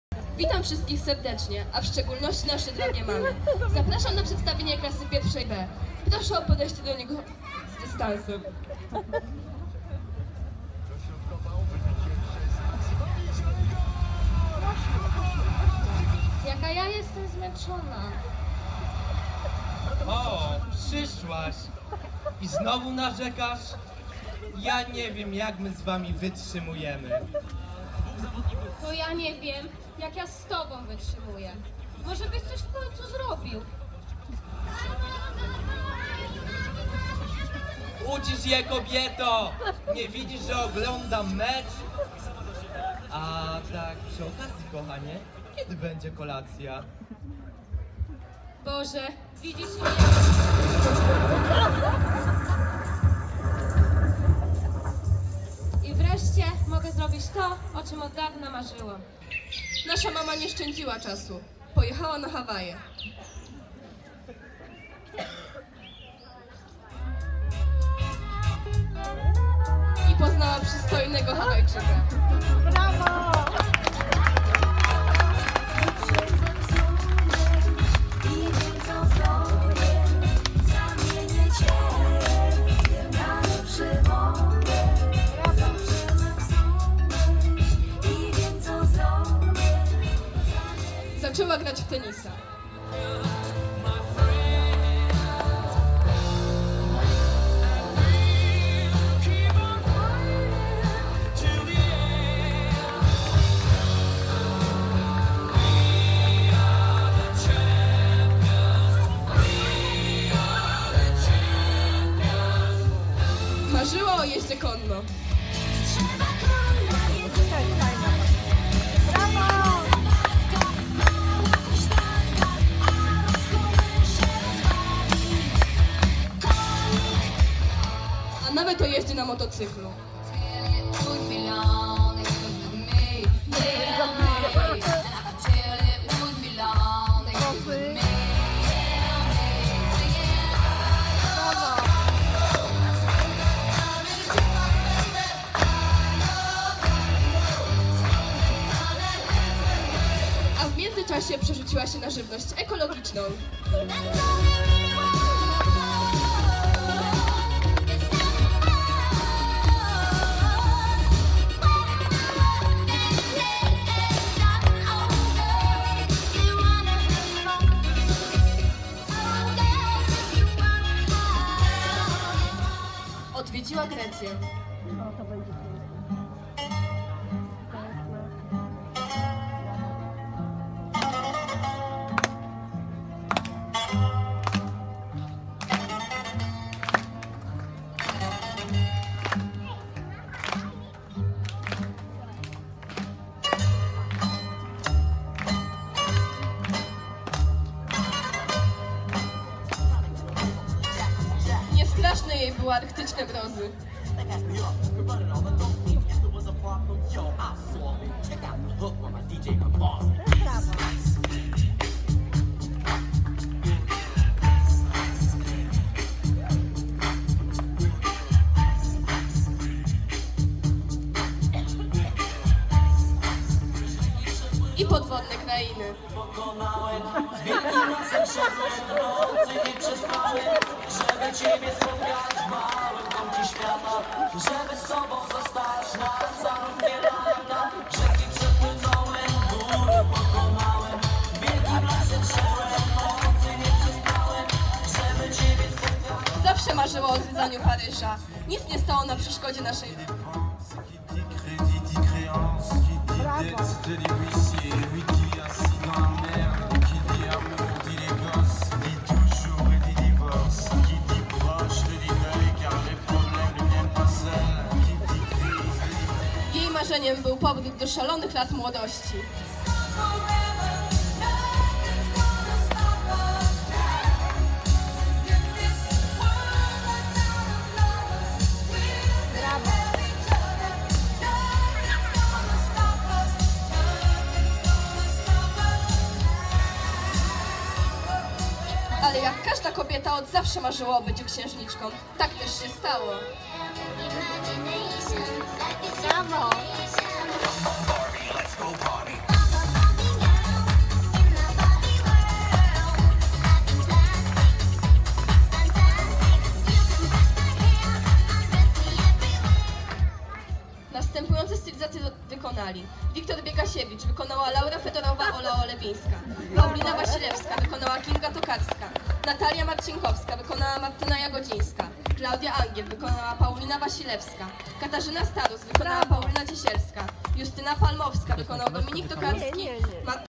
II Gala Dyplomowa Anagry - Klasa 1b
4 czerwca 2016 roku odbyła się II Gala Dyplomowa Łódzkiej Szkoły Mody Kosmetologii Fryzjerstwa Anagra